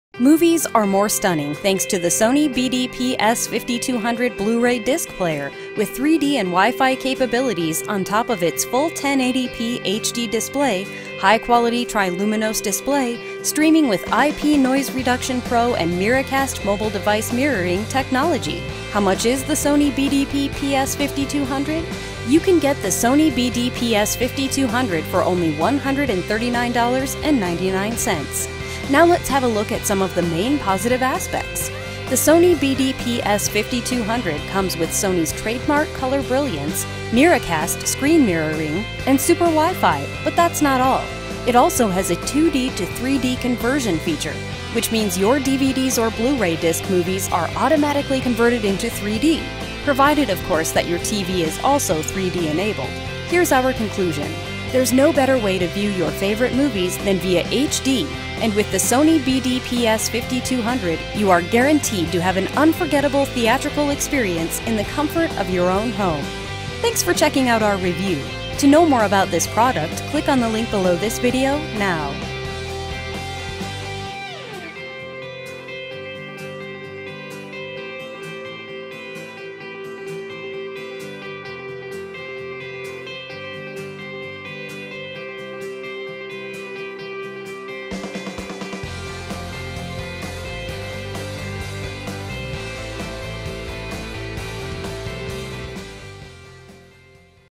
Bdps5200 Blu ray
the Sony BDPS5200 Blu-ray Disc Player with 3D and Wi-Fi, one of the latest in Sony's line of outstanding Blu-ray disc players. It features full HD 1080p Blu-ray disc playback and also upscales the picture quality of your regular DVDs to almost HD quality. Movies on DVDs and Blu-ray discs are also automatically converted to 3D during playback on a 3D TV, thanks to this player's 2D to 3D conversion feature. That means you can enjoy more stunning detail and clarity on the movies you are watching. It also has a high-quality TRILUMINOS display, as well as a Quick Start/Load function that lets you watch movies much faster.

With the Sony BDPS5200, you can easily stream more than 200 services, and you don't have to suffer through low quality streaming content, either. Its IP Noise Reduction Pro technology effectively improves streamed content so you can still watch it in HD. You literally have the Sony Entertainment Network within easy reach, and you can also fully utilize apps, such as the My Sports & Fitness app, perfect for couch potatoes who deserve a break from watching once in a while. Streaming is made possible by this unit's super Wi-Fi capability.

Sony BDPS5200's super Wi-Fi also works in tandem with Miracast screen mirroring technology in order to let you mirror your mobile device directly on your TV screen. Throw media from your Sony Xperia Tablet, stream photos, videos and music using DLNA, or simply plug your media storage device via this unit's USB slots, and enjoy watching photos and videos or listening to music. Its built-in Wi-Fi also lets you socialize with other viewers on social media platforms like Facebook and Twitter. Once connected to the internet, you can also use your Blu-ray disc player to access and search information on movies using Gracenote services.